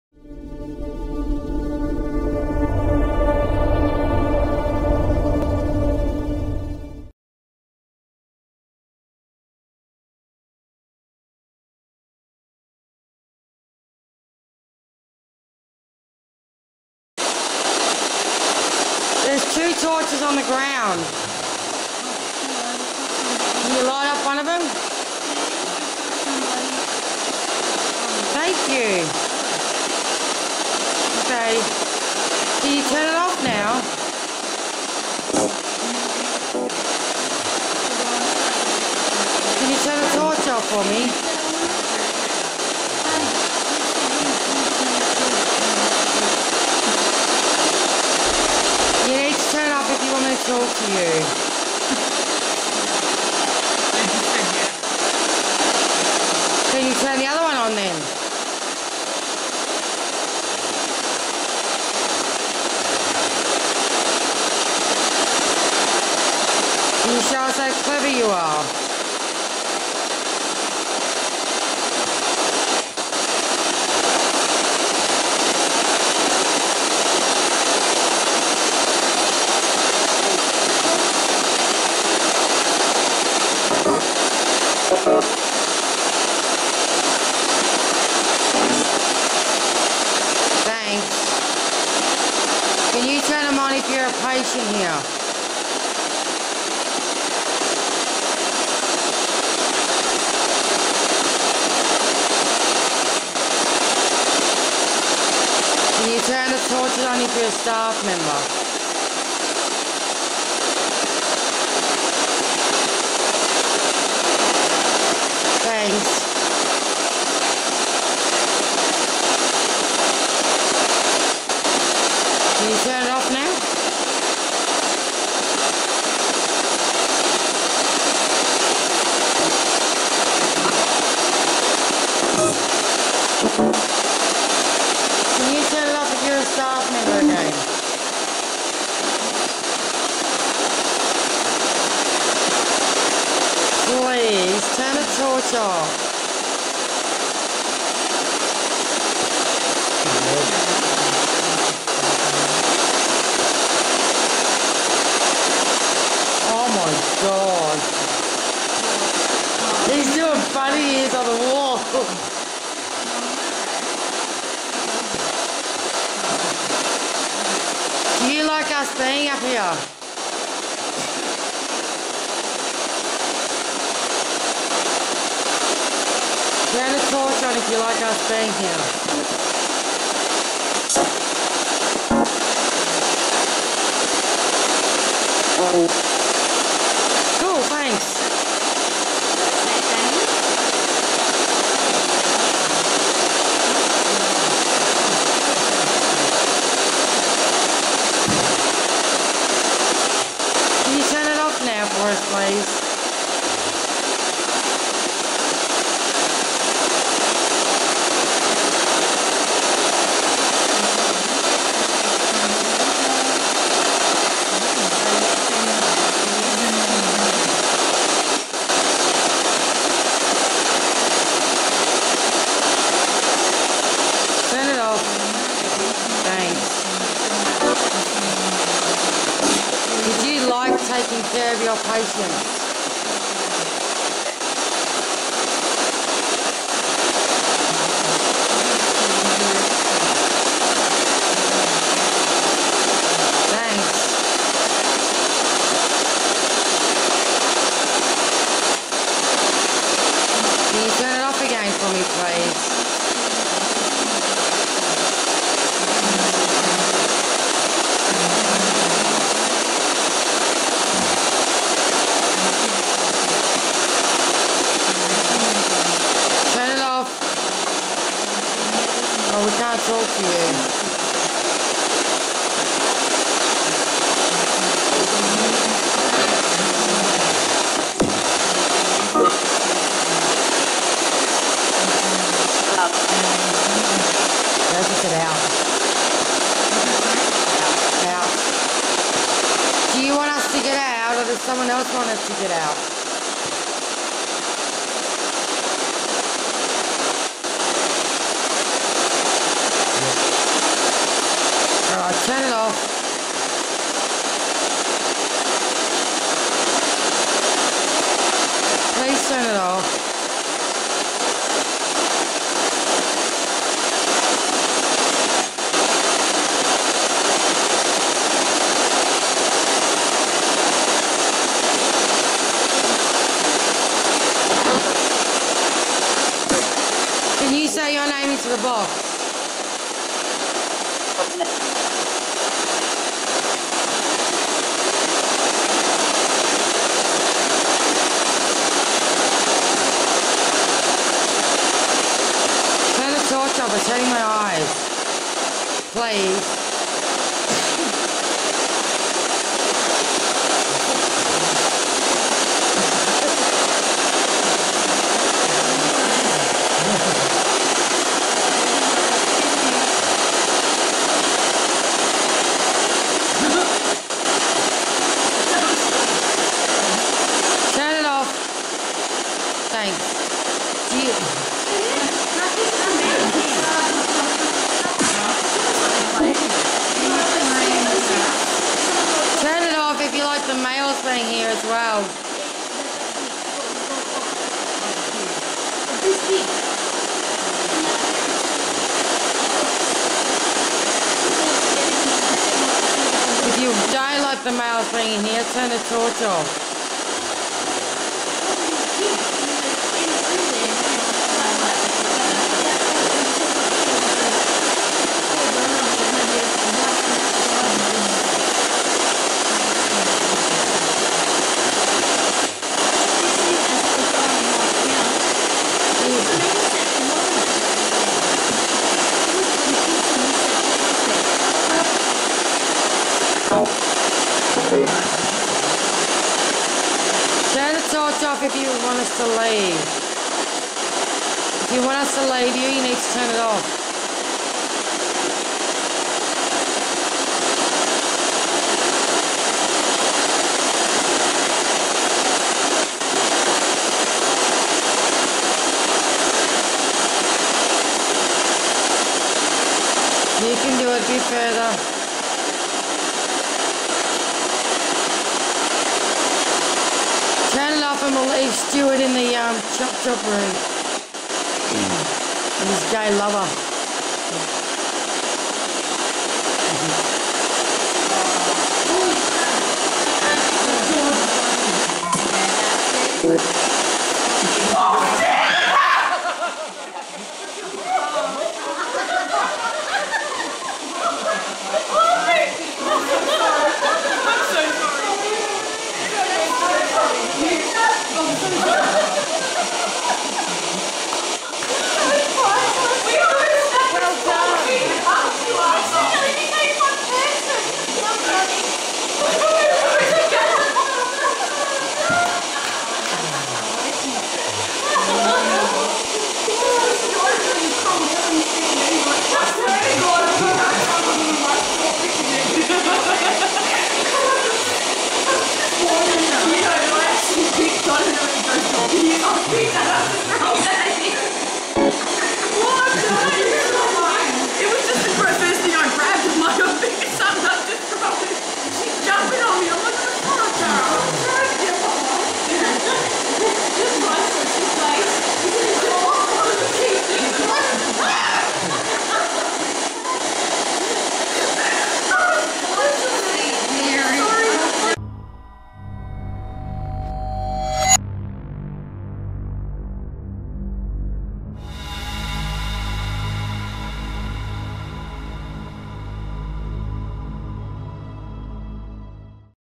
25/10/2014 Friends Sleepover
Course Language,friends of mine doing a sleepover lol